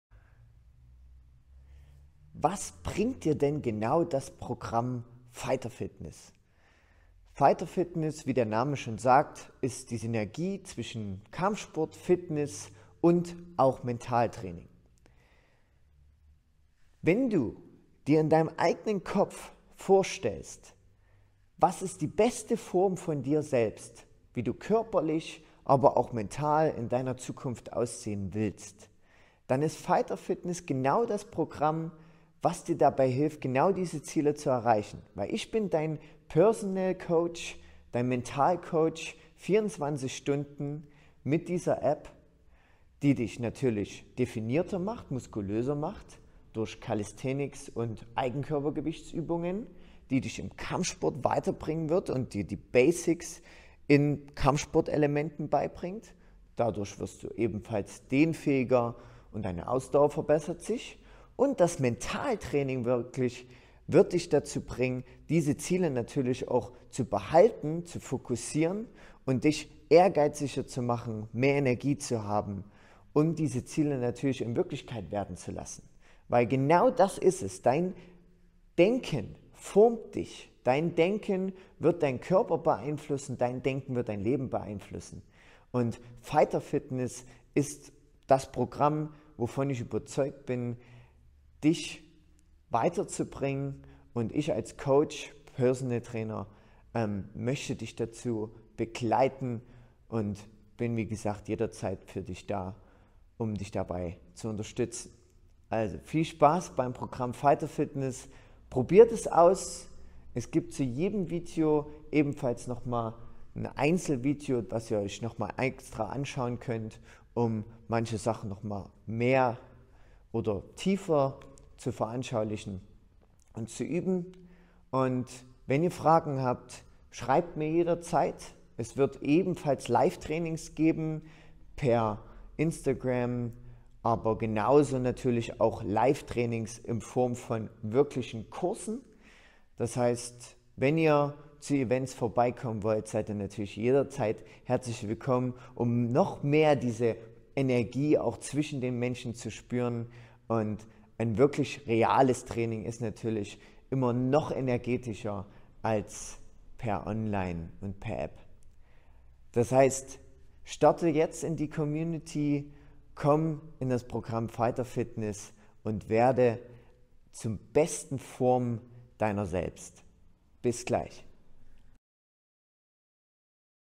Was bringt dir FIGHTER FITNESS?
Du wolltest schon immer mal Kampfsport probieren?
Dann ist FIGHTER FITNESS dein perfekter Einstieg! Downloade dir einfach die FIGHTER FITNESS APP und starte noch heute mit dem Training!
Für Android:
Hier hast du die Möglichkeit, immer und überall an deiner Fitness zu arbeiten. Trainiere allein und individuell mit der App UND gemeinsam mit uns im Team in unseren regelmäßigen Live-Workouts. Bei uns geht beides. Mit motivierendem Video-Content und einer großen Auswahl an vielfältigen Übungen, ermöglicht dir Fighter Fitness die perfekte Einbindung in deinen Alltag. Ganz gleich ob du Anfänger bist oder dich schon auskennst, hier findest du alles, was du für ein vollwertiges Workout brauchst. Kampfsport, Kraft, Beweglichkeit und Mindset sind nur eine Auswahl unserer Bereiche, in der dich Fighter Fitness stark macht.

Starte jetzt mit uns durch und bringe deinen Körper und deinen Geist auf ein völlig neues Level! Mit dem Premium-Abo stehen alle Übungen und die Live-Workouts für dich zur Verfügung.

Körper & Geist:

Neben unseren Workouts für Anfänger und Fortgeschrittene gibt es Kampfsportübungen aus den Bereichen Functional Training, MMA und Kickboxen.
Wir steigern die Beweglichkeit deines Körpers und stärken deine Körperkraft. Gezielte Warm-up & Cool-down-Übungen runden dein perfektes Workout ab. Außerdem erfährst du, wie du an deinem Mindset arbeiten kannst. Lerne deine Atmung zu kontrollieren und Stärke nicht nur deinen Muckis, sondern auch deinen Geist.

Maximale Freiheit & größtmögliche Individualität:
Neben unseren Videos in der App gibt es regelmäßige Live-Trainings, in denen dich unsere professionellen Fitnesstrainer ganz individuell da abholen, wo du dich gerade befindest. Und wenn du neben der Trainings-App einen Coach brauchst, zum Beispiel wenn du körperliche
Einschränkungen oder Vorerkrankungen hast, dann nimm einfach direkten Kontakt mit unseren Fitnesstrainern auf und lass dir ein ganz auf dich zugeschnittenes Workout definieren.